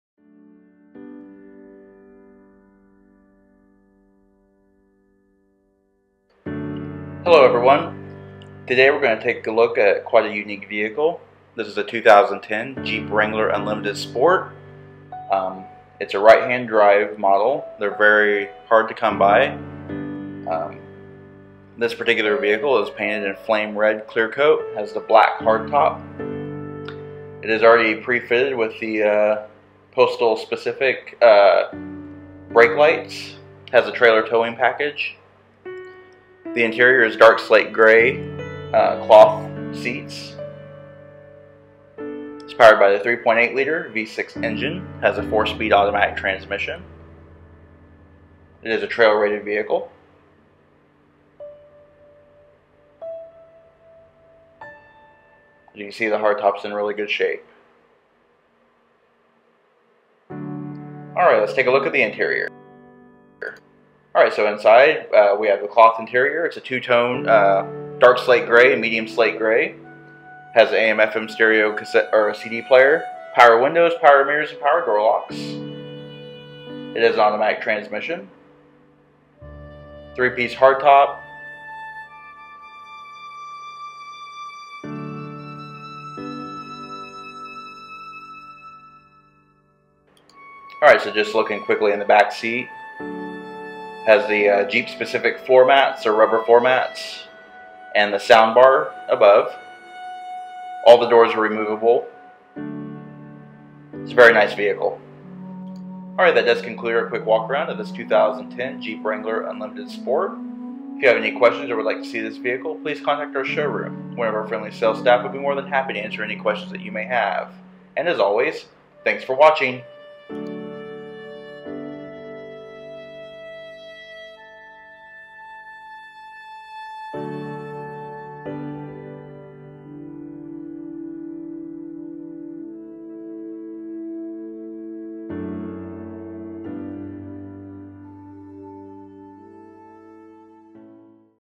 2010 Jeep Wrangler Unlimited Sport Right Hand Drive | Indianapolis, IN area
Postal workers rejoice! This is a right hand drive Jeep. Very hard to find and perfect for that mail route or other delivery positions.

Located at:
Community Chrysler
555 State Rd. 37 S.
Martinsville, IN